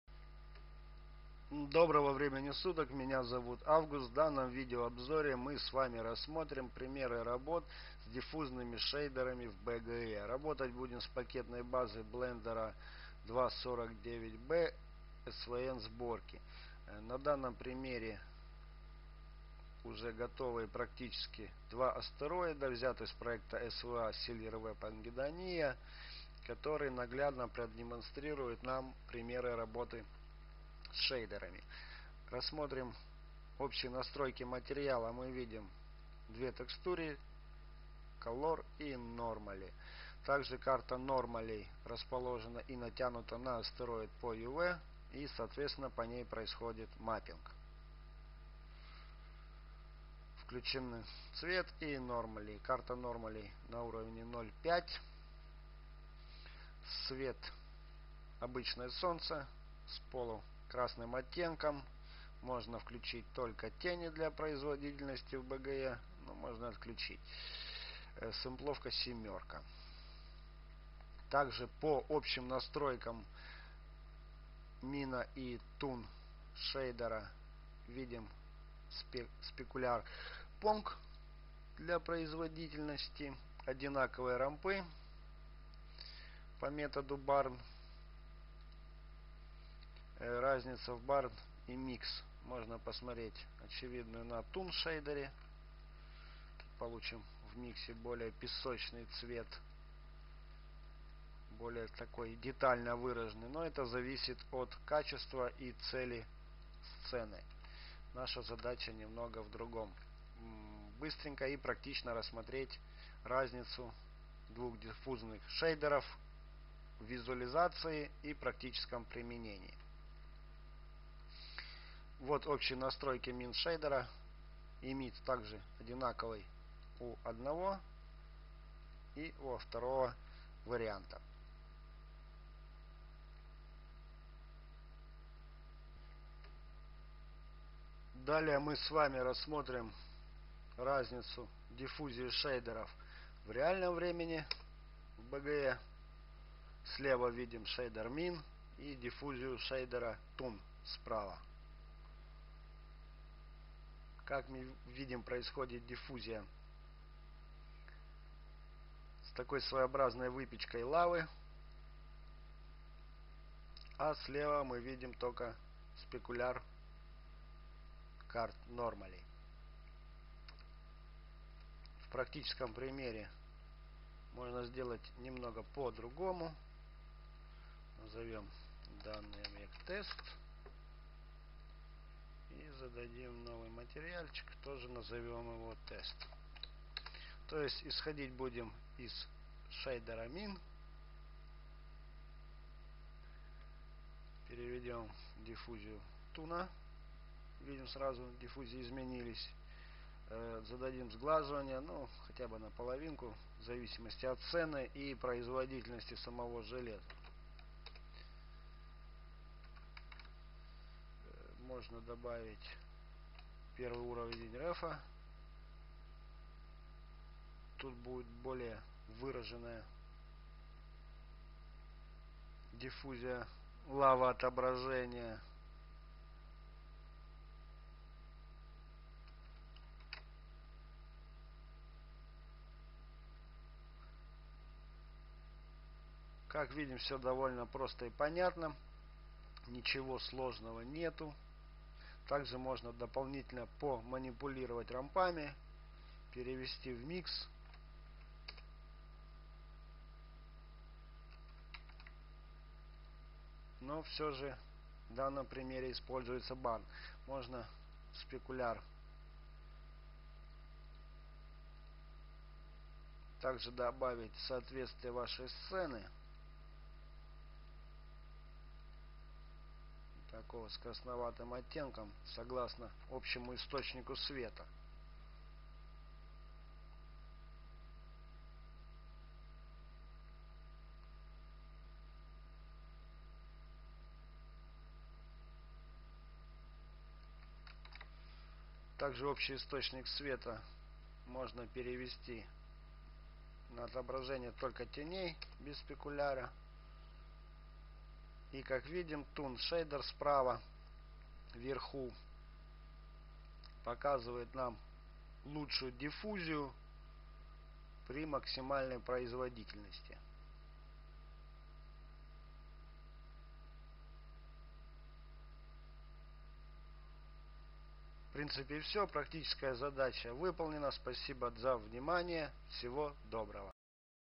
Blender3D BGE / Minnaret vs Toon
This article describes a simple video example of the use of deficit
Shader BGE.

It is based on asteroids from the project SWA.
PyNodes not used.
Reviewed by the general course material settings.

Example for Blender3D 2,49 b. ..